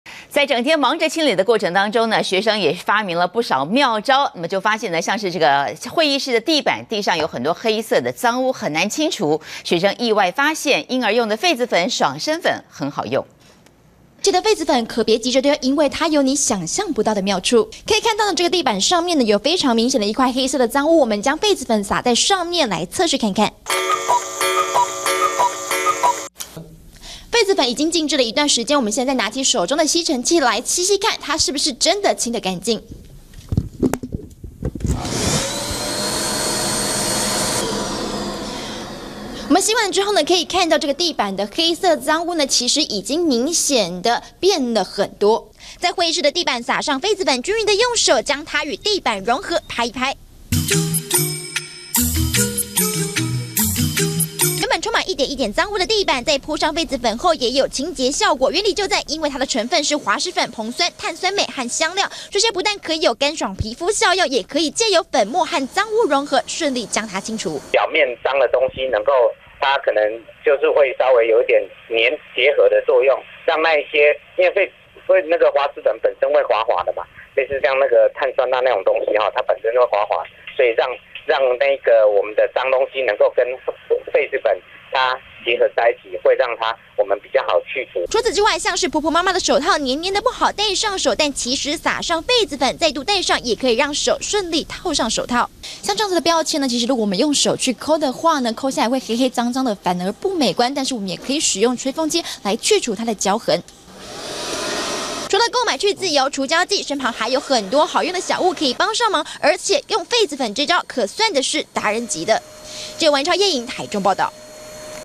20140410中天新聞 痱子粉別丟 會議室地毯清理好幫手
嬰兒用的痱子粉和爽身粉，除了拿來擦身體，現在也可以拿來清理會議室的地板，難以清理的黑色髒污，藉由和粉末的結合，可以順勢將污漬去除。